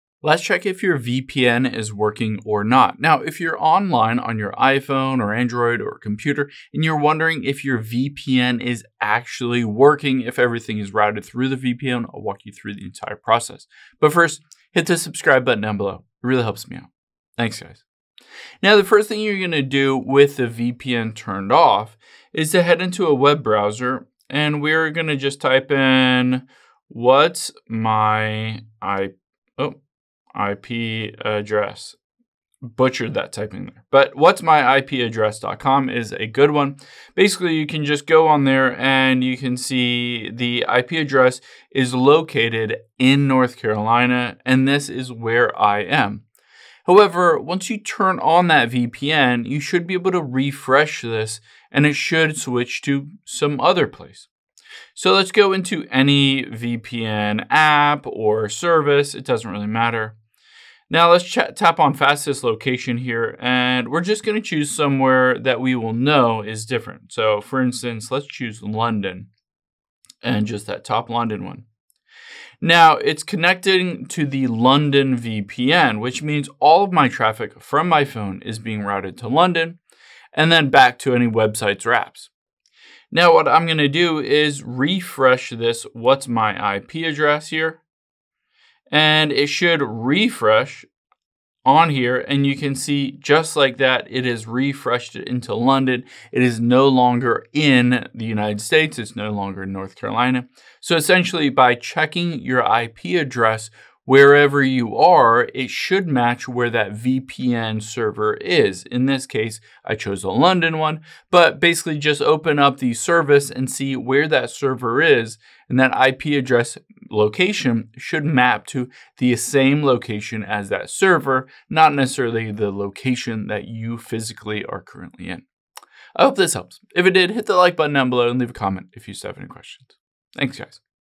How To Check If VPN Is Working Or Not
Here's how to check if a VPN is working or not so you can know if your location and IP address is properly hidden.